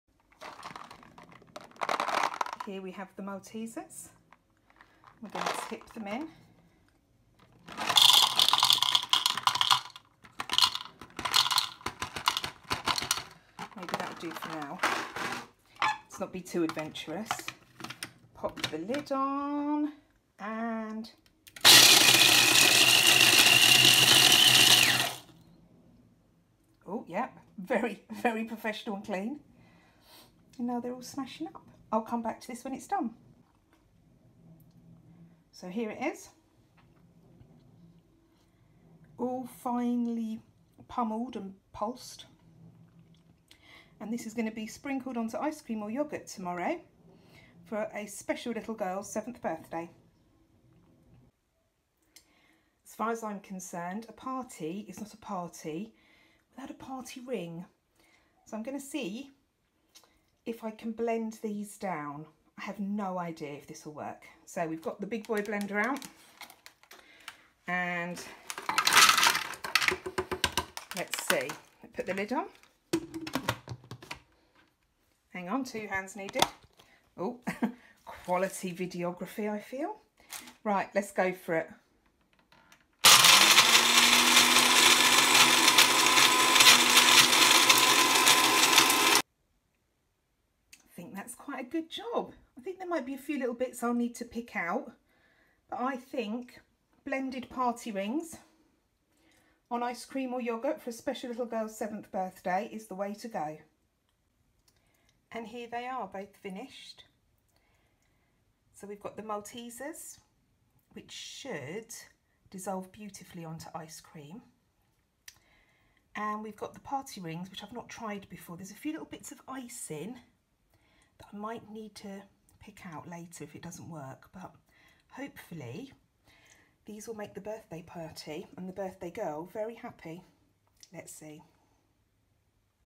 Party Rings are essential
A special little girl was turning 7 and I wanted to ensure she had full access to proper party food.  By 'proper party food' I'm obviously meaning maltesters and party rings - goes without saying, right?

I'm trying to blend different foods to see how they turn out.  These went better than expected to be honest.
However. my videoing skills and editing inability remains low. so good luck! I'm going to try really hard to get better although I suspect this will take time.

Happy blending